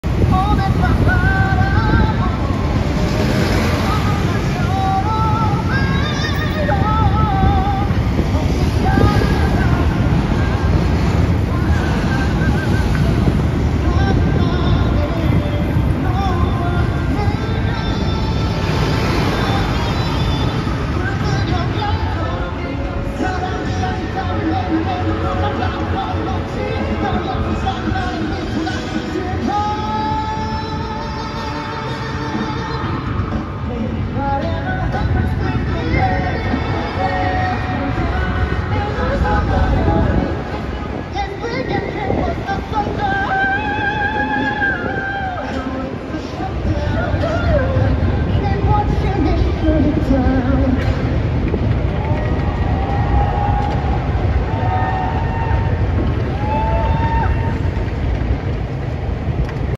BOYS PLANET イ・フェテク 、日本ユニカビジョン大型電光掲示板で上映されている映像···「ファンたちの応援が日本まで！」
[WoWKorea 이상철 기자] 10일 오후 일본 도쿄도 신주쿠에 위치한 유니카비전 전광판에서 Mnet '보이즈플래닛' 연습생 이회택을 응원하는 특별 영상이 상영되고 있다.

[WoWKorea イ・サンチョル 記者] 10日午後、日本東京都新宿に位置したユニカービジョン電光板でMnet「BOYS PLANET」練習生「イ·フェテク」を応援する特別映像が上映されている。